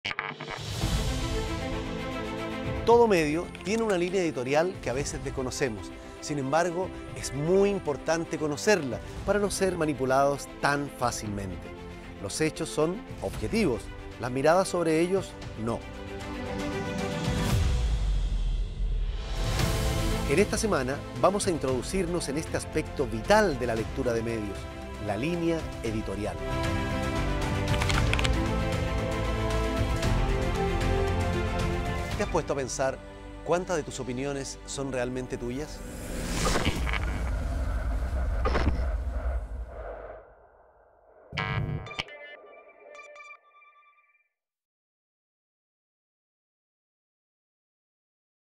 Semana 2 , Manos a la Obra [Curso Conflictos del siglo XXI en Latinoamérica]
Se abre la segunda semana del curso, a la que hemos llamado “Lo que callan los medios”, que tratará sobre la identificación de la línea editorial y hacer la diferencia entre la objetividad de los hechos y la subjetividad a la hora de tratarlos.